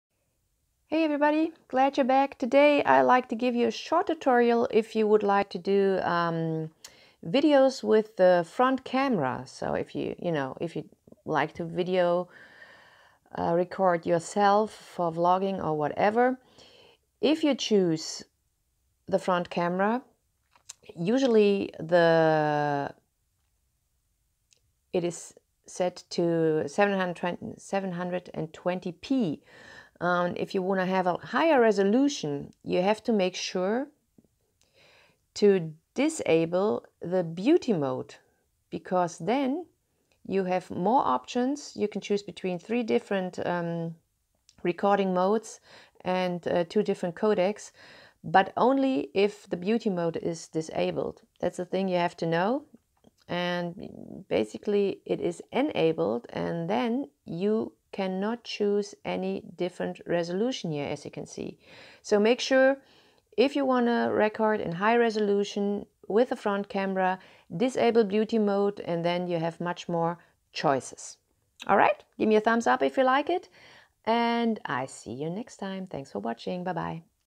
Huawei P20Pro: Changing the video resolution of the front camera
Easy trick, how you can choose a higher resolution for the front camera